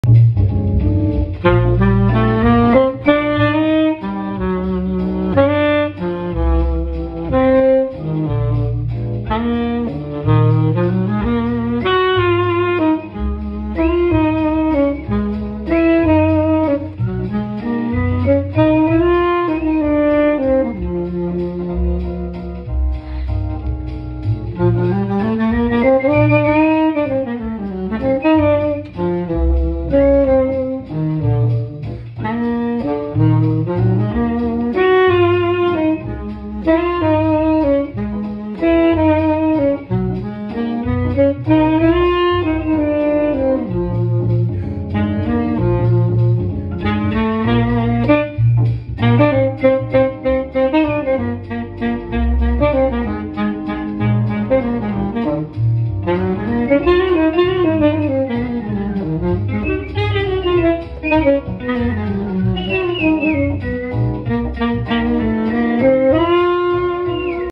Toots Thielemans short transcription
I love this song! Will be learning the full transcription over the next few days!